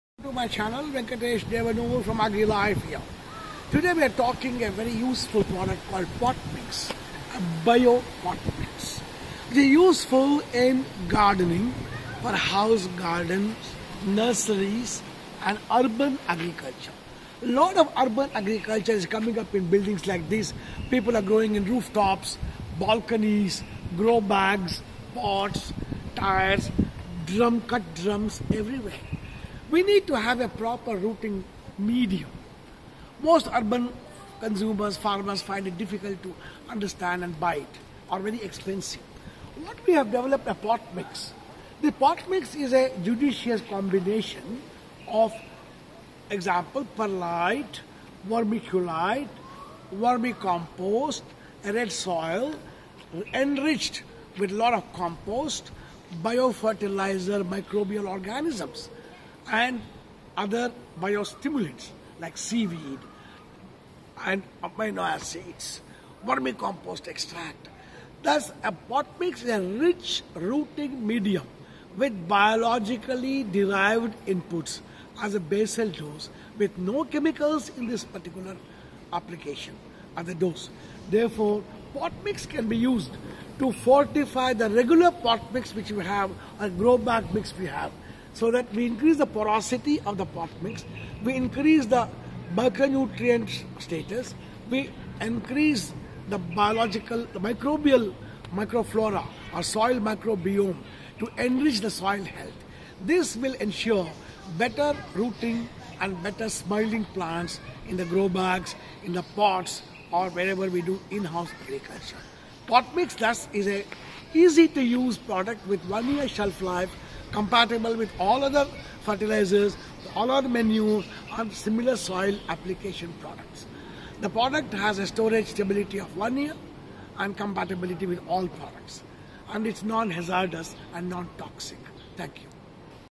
Potmix - Enriched rooting medium
The Potmix is a combination of perlite, vermiculite, vermicompost, soil, enriched with biofertilizers and biostimulants like seaweed and amino acids. This can be used to enrich the regular potting mix or as stand-alone to aid in better rooting of potted plants.